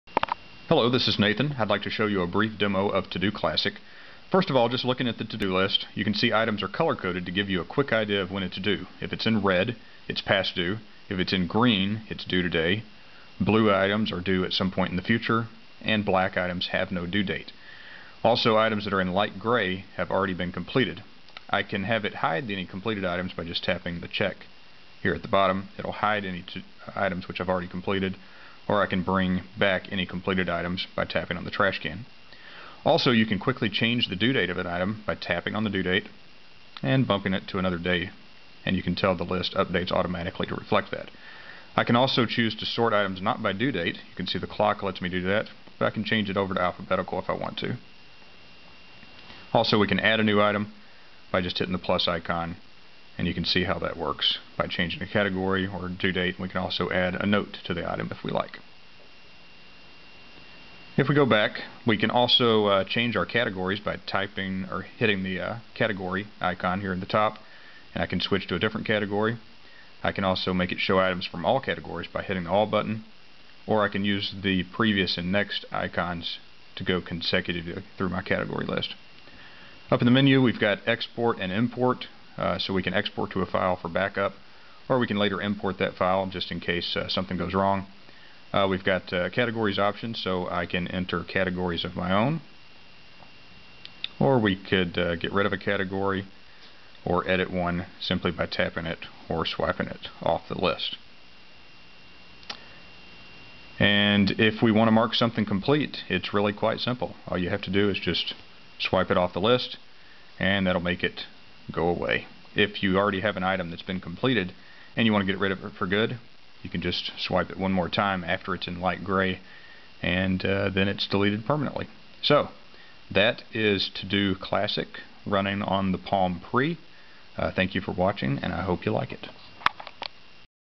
ToDo Classic demo on Palm Pre
This is a demo of the ToDo Classic app running on the Palm Pre.  and look for it in the Palm App Catalog.